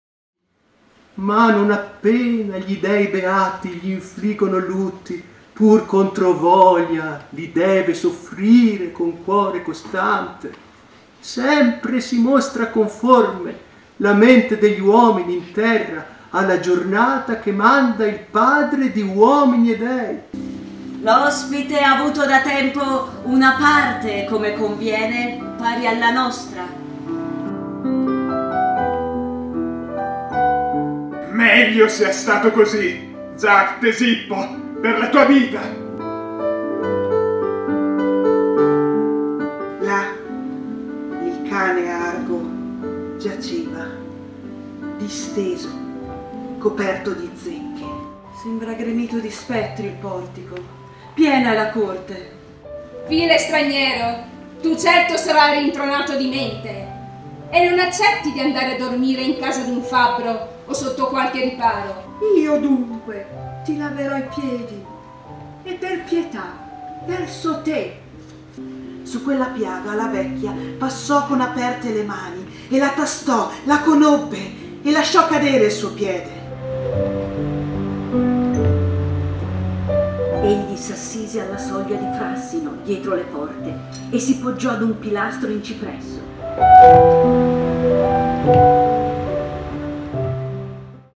IN VIAGGIO CON...L'ODISSEA: UN CUORE COSTANTE. REPLICA | 2019
XI appuntamento con il Format "In Viaggio con..." dedicato quest'anno alla scoperta del poema epico l'"ODISSEA". Titolo: Un cuore costante. Odisseo mendicante nella reggia. Cunevo | Teatro Comunale. Format ideato e promosso dalla Comunità della Val di Non con la collaborazione di: Comuni, Associazioni e Istituti di Credito del territorio.